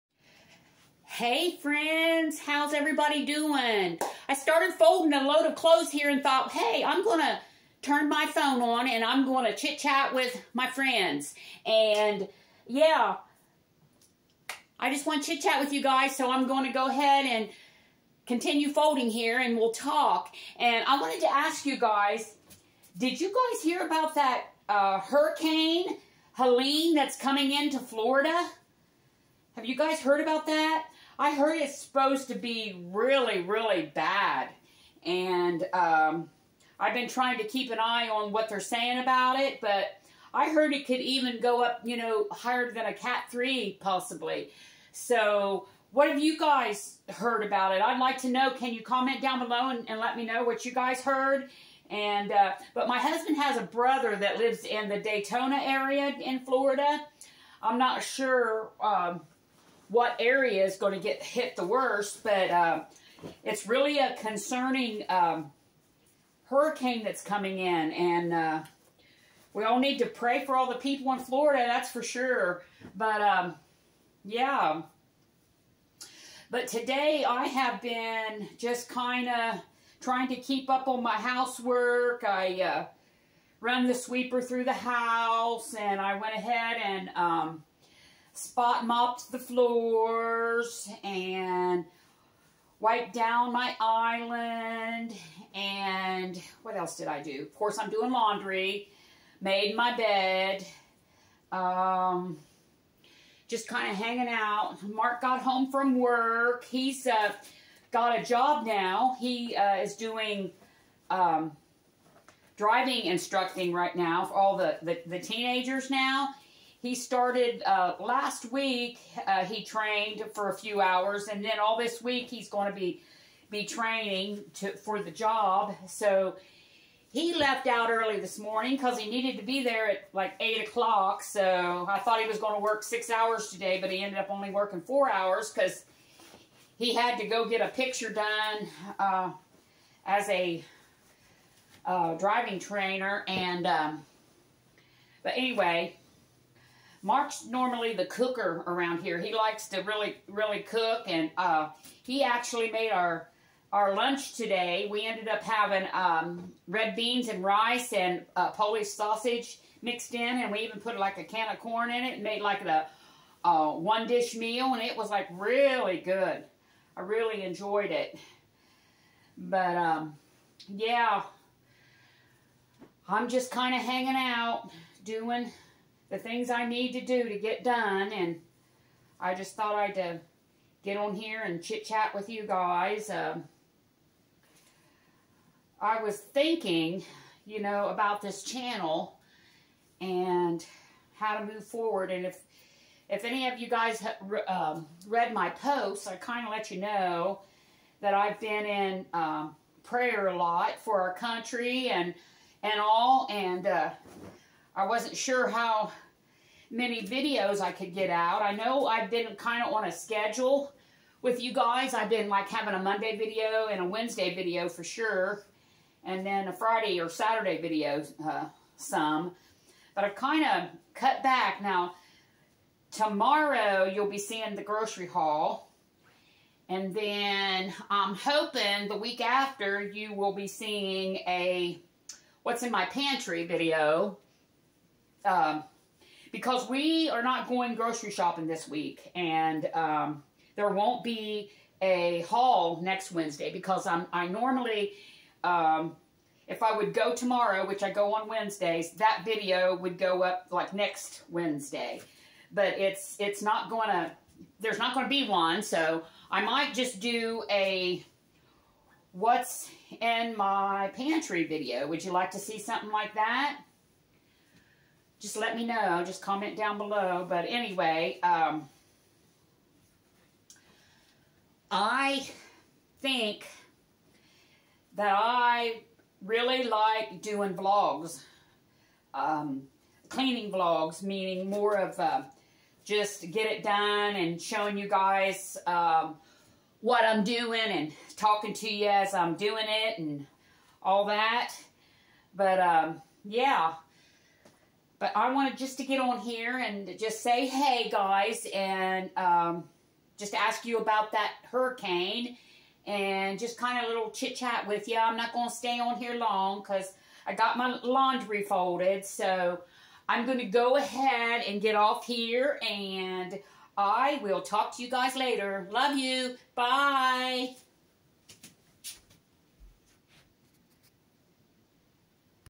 chit chat with my friends
I thought while I was folding a load of clothes I would chit chat with you. Blessings